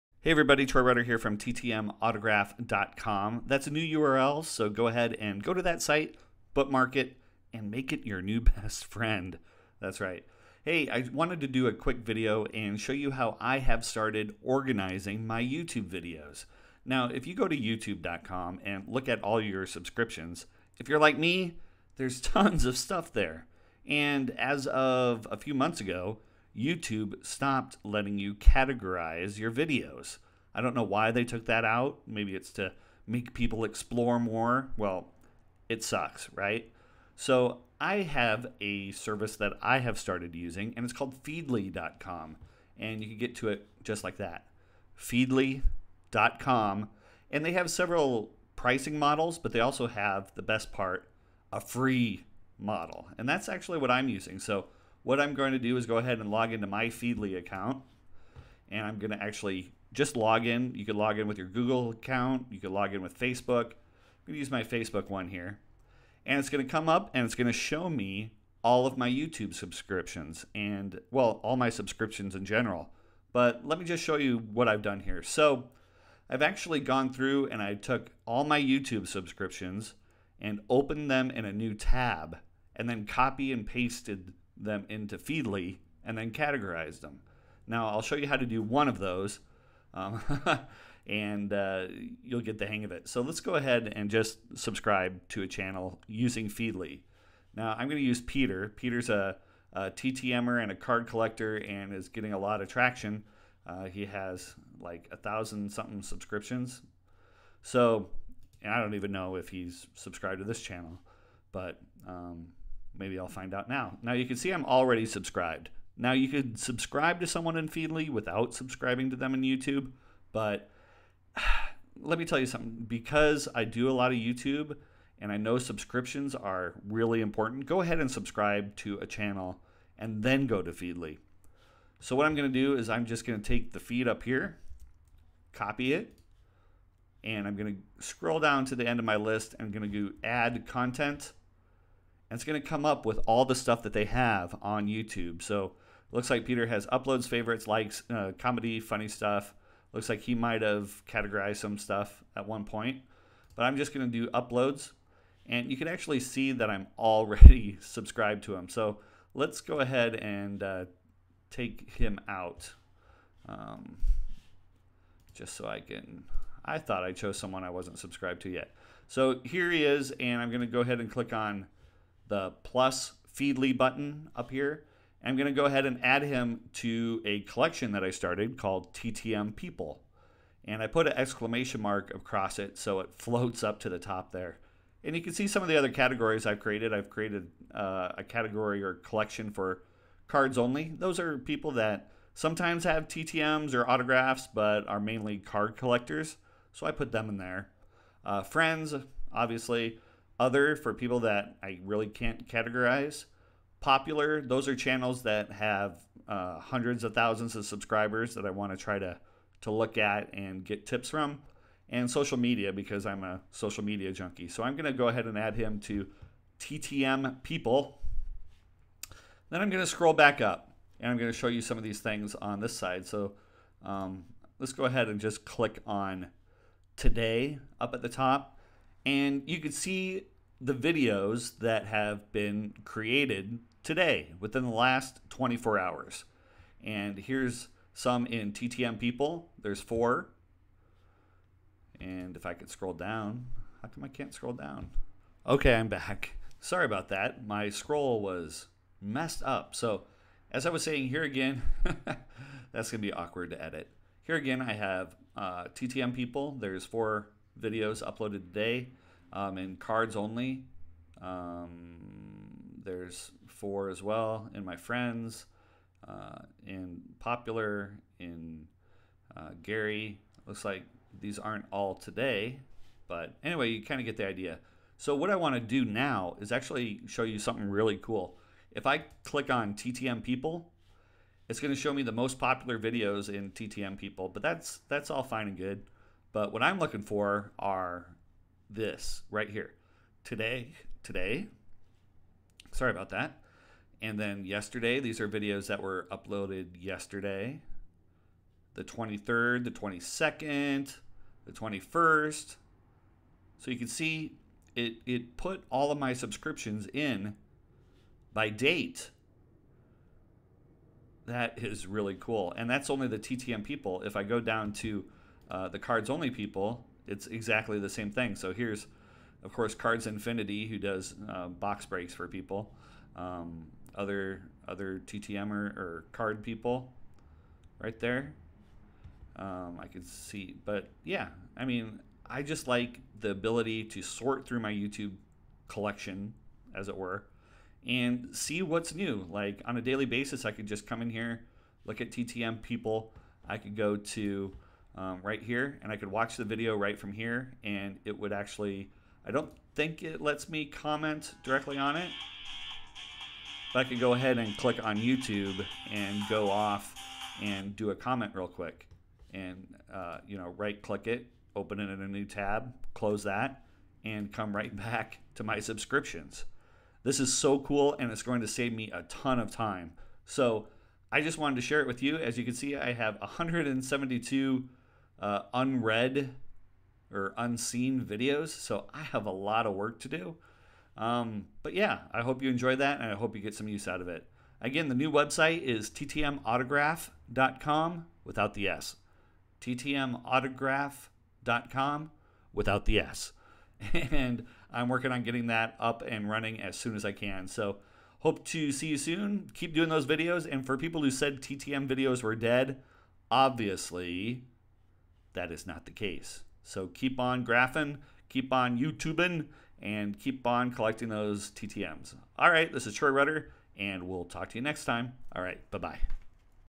Organizing Your YouTube Subscriptions - Save Time!
Are Your Youtube subscriptions getting hard to handle? Mine are! Since YouTube got rid of the categories feature, everything is a mess - luckily I have found a way to not only keep track of them, but but also see them by date, creator, whatever! Need to see what all was uploaded today? It's easy!

soon!